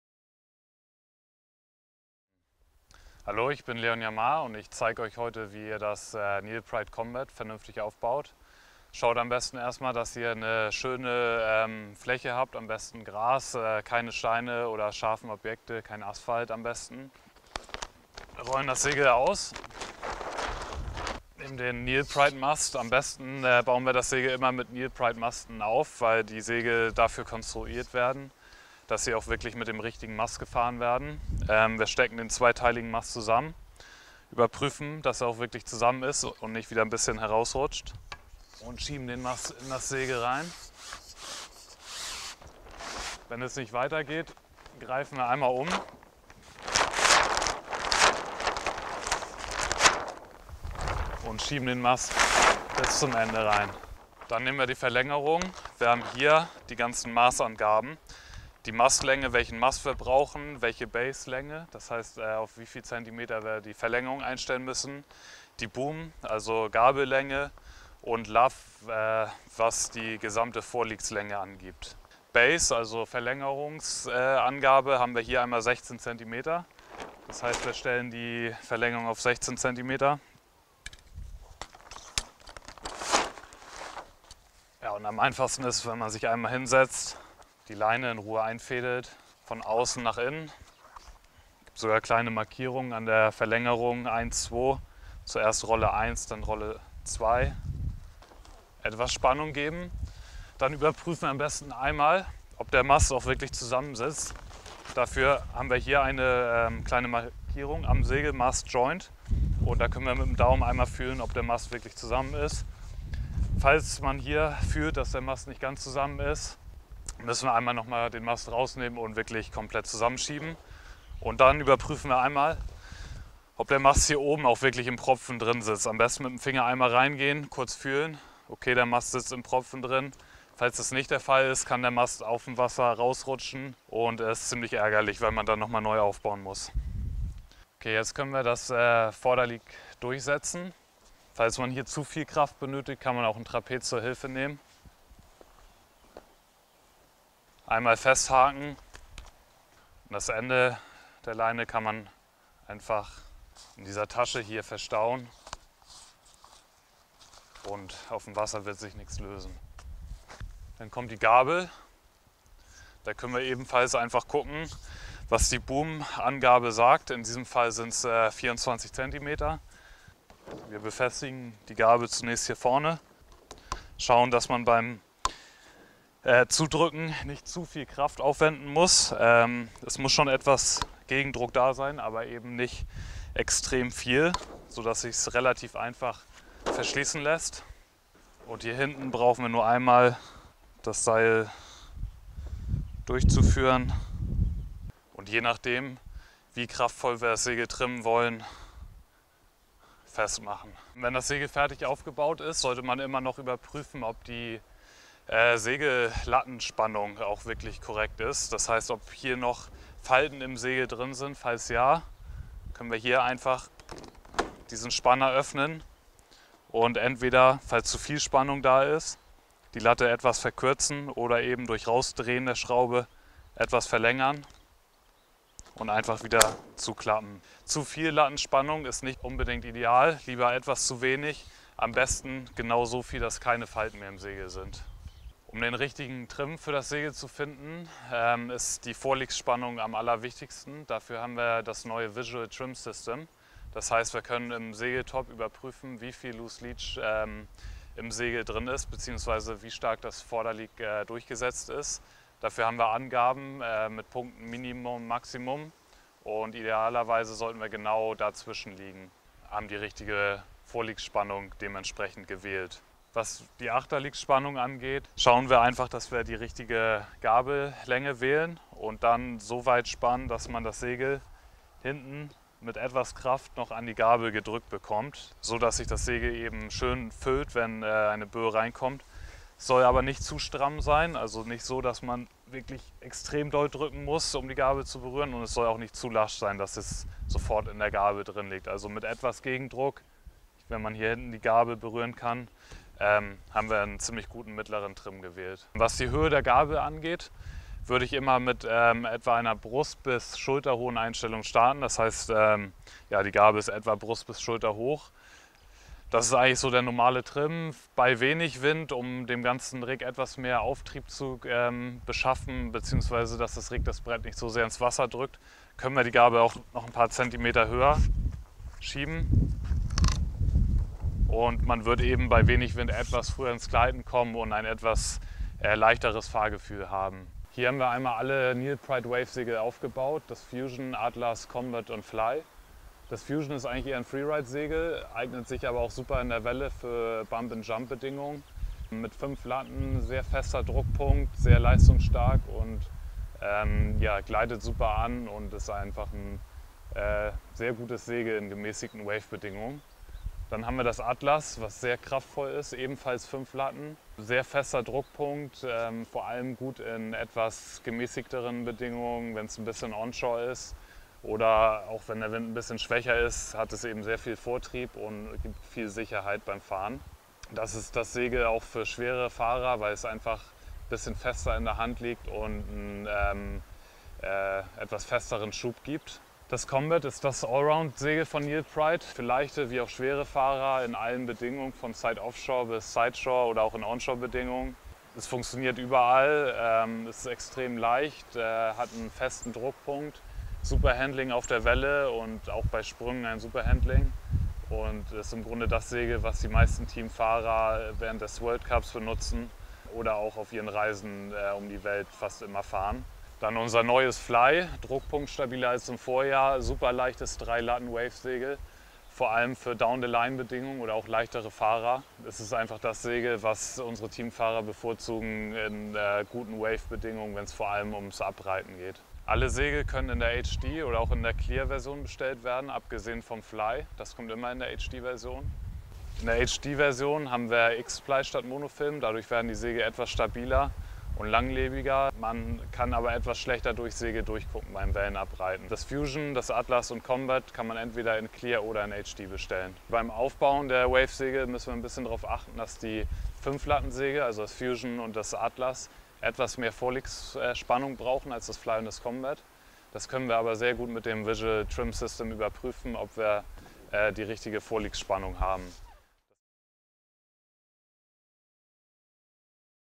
2018 NeilPryde Wave Segel Aufbau und Tuning
Leon Jamaer erklärt dir, wie du dein NeilPryde Wave Segel am einfachsten und besten aufbaust und gibt dir außerdem Tips für den richtigen Trim, damit du die best mögliche Performance des Segels auf dem Wasser genießen kannst. Außerdem gibt Leon dir einen Überblick von der gesamten Freeride-Segel Range.